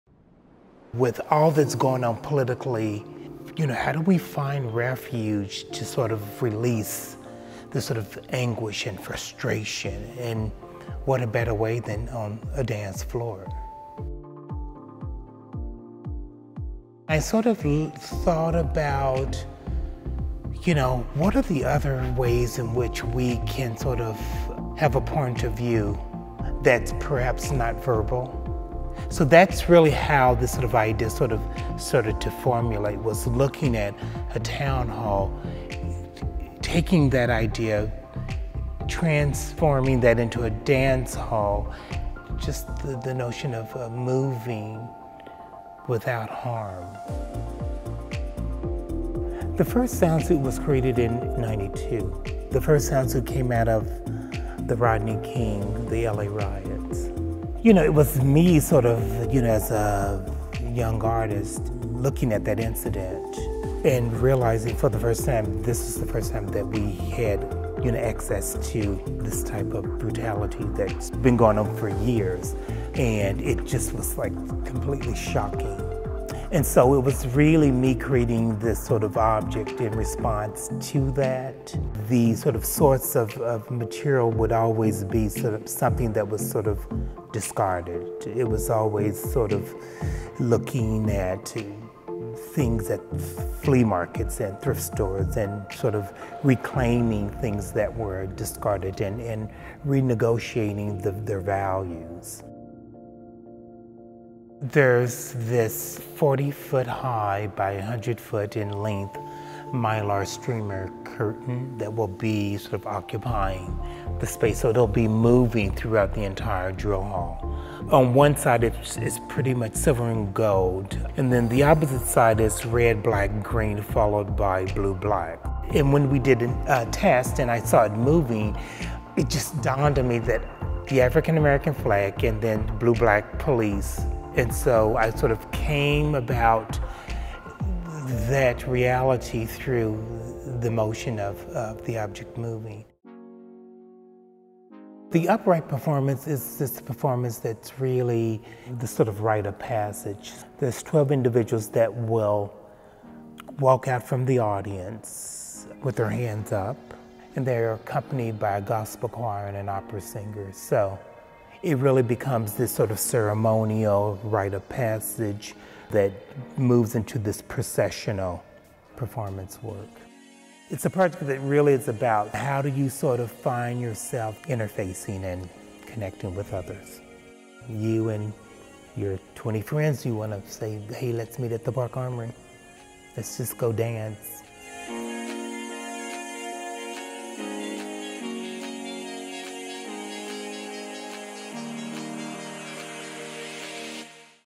Nick Cave's "The Let Go" at the Park Avenue Armory
The conceptual artist Nick Cave discusses his Park Avenue Armory installation "The Let Go," a town hall-inspired dance hall that is part performance, part immersive experience, part dance party.